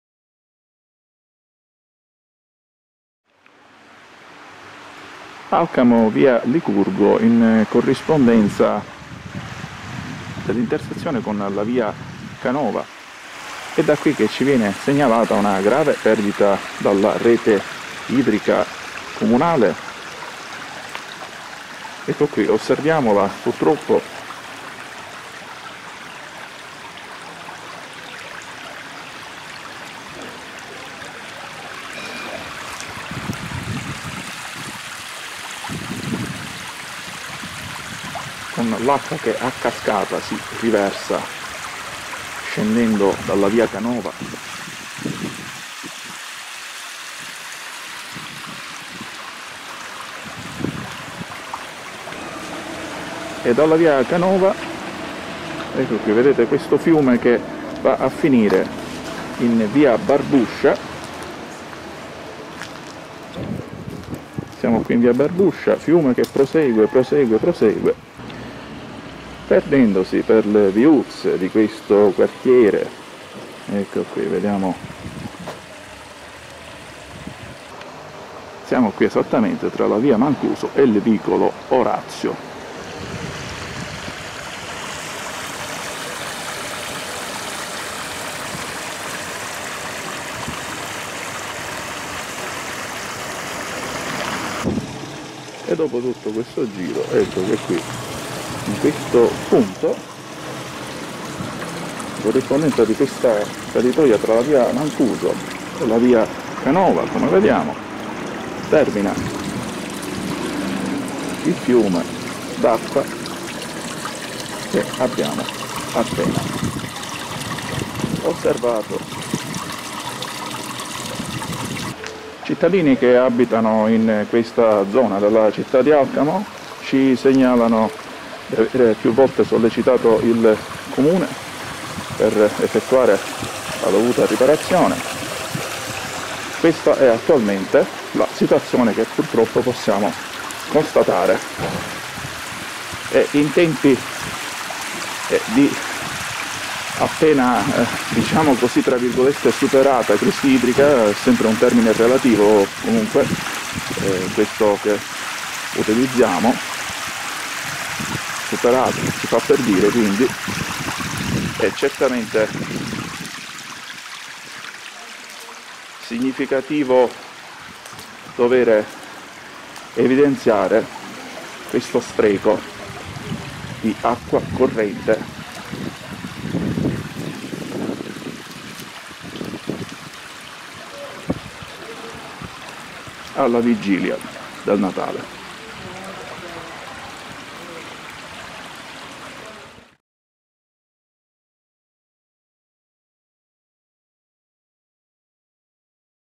Alcamo e le sue perdite d'acqua. Dalla via Licurgo un fiume con le cascate
Venerdì 24 dicembre 2021. A cura di Massimo Provenza - Un impressionante quantitativo di acqua corrente, come potete osservare in questo servizio filmato, si perde lungo le strade del quartiere situato alle spalle dell'Ipab "Mangione". Abitanti nella zona hanno più volte sollecitato il Comune ad intervenire per riparare la falla.